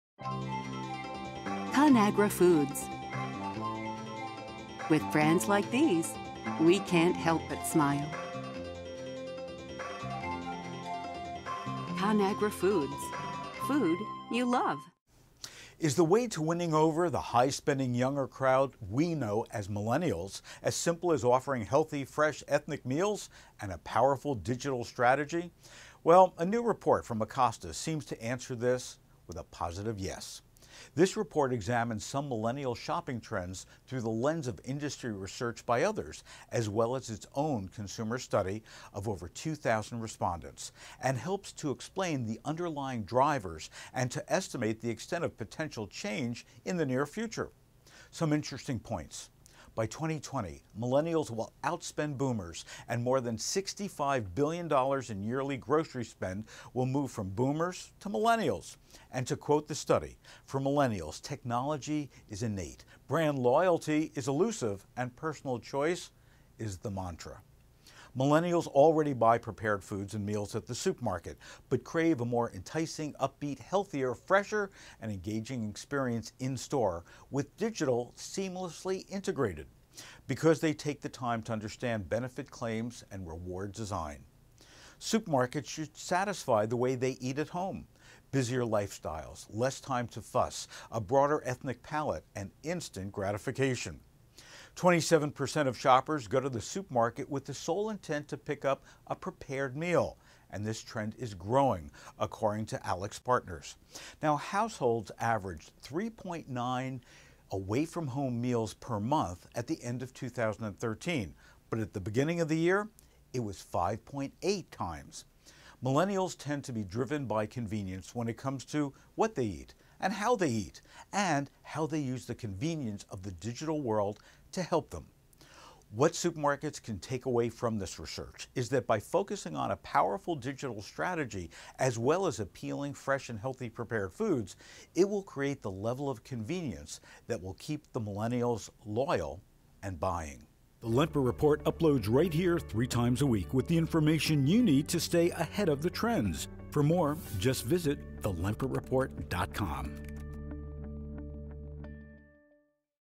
The way to a millennial's heart
A new report from Acosta examines some millennial shopping trends through the lens of industry research by others.